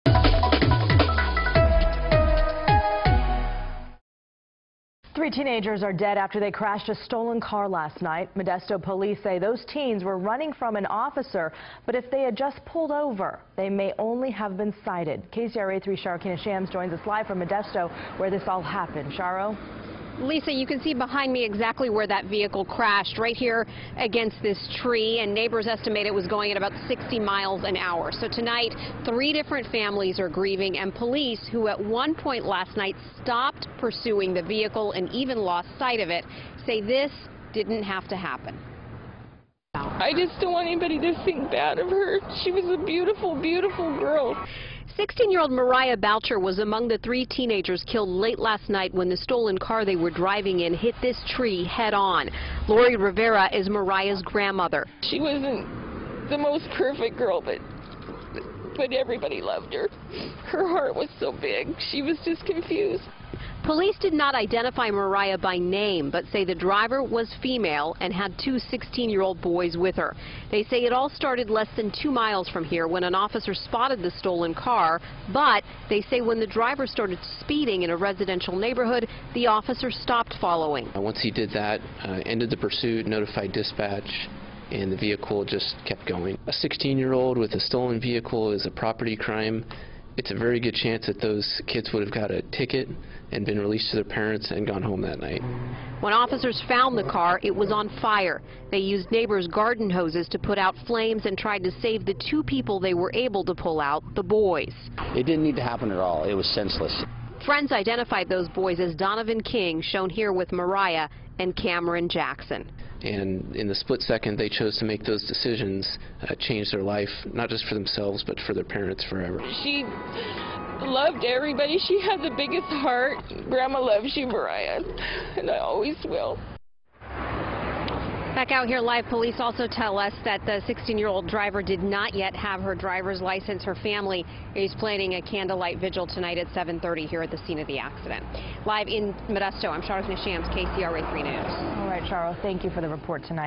3 Modesto teens avoid traffic stop, die in crash
Three teens in a stolen vehicle crashed into a tree as they tried to evade police in Modesto, officers said Friday.